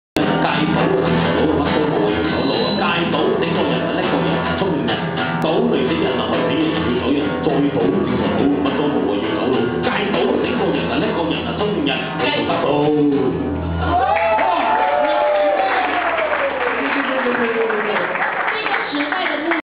林万兴牧师rap歌助兴
《谁是赢家？》座谈会
2012年02月16日，星期四
槟城光明日报总社